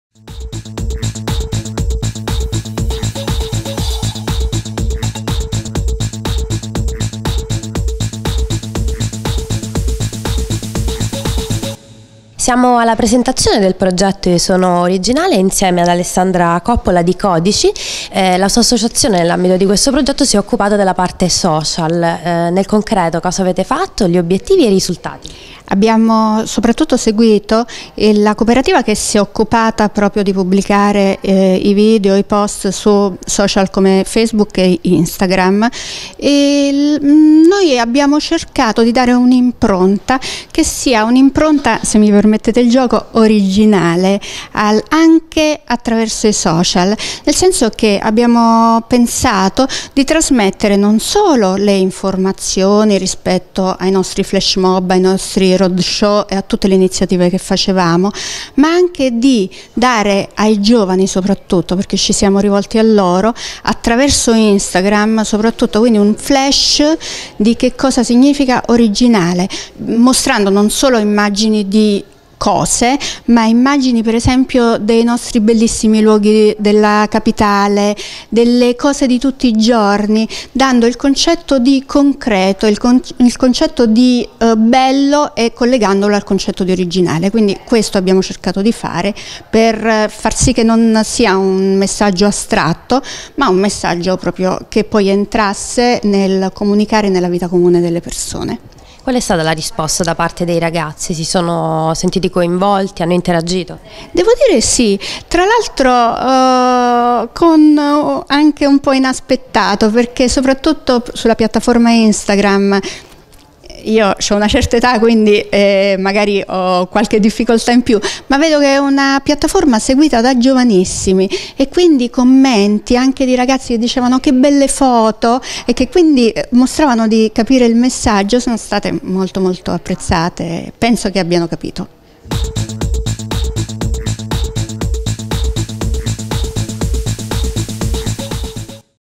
Io sono originale, intervista ad Alessandra Coppola, CODICI
Presentazione del progetto IO SONO ORIGINALE
Nella giornata di giovedì 15 giugno, nell’ambito della settimana per la lotta alla contraffazione promossa dal Ministero dello Sviluppo economico, Direzione Generale Lotta alla Contraffazione – Ufficio Italiano Brevetti e Marchi , sono state presentate le attività svolte nell’ambito del progetto “Io sono originale”